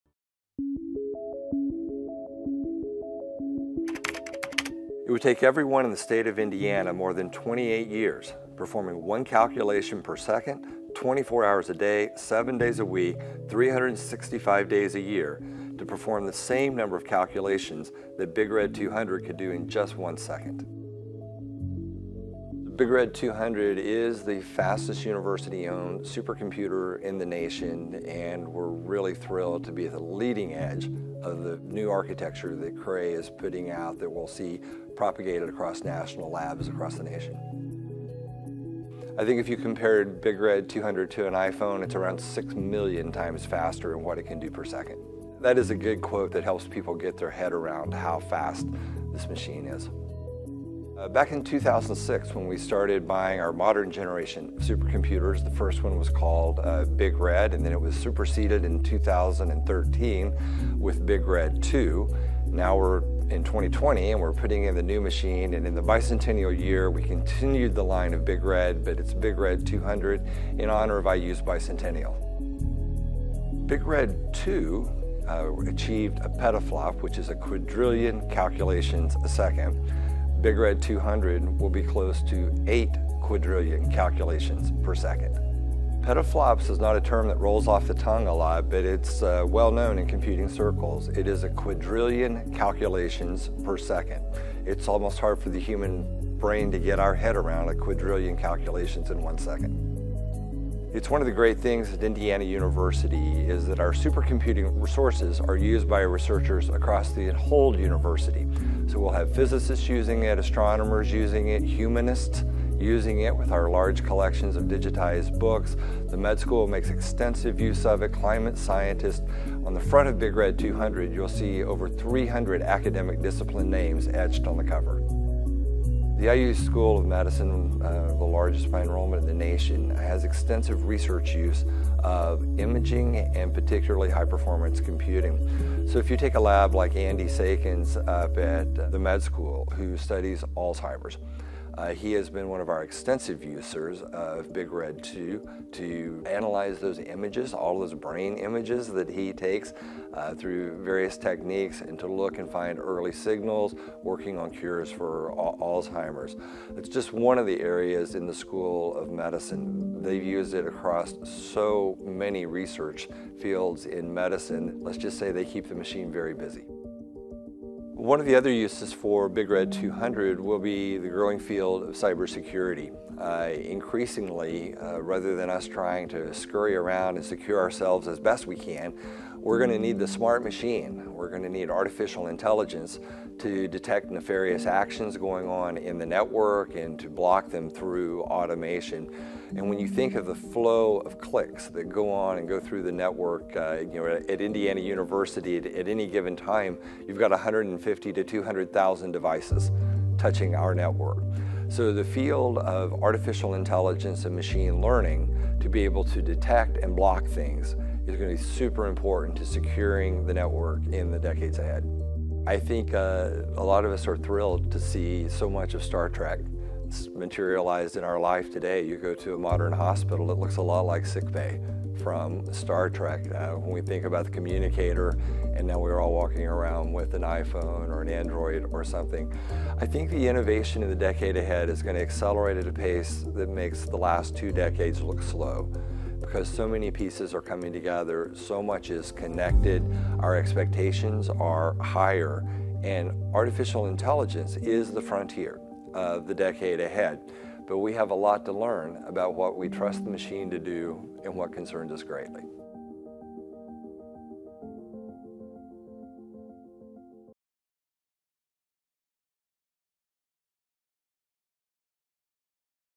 Journey Indiana - Big Red 200
Learn all about Big Red 200, Indiana University's latest - and greatest - supercomputer.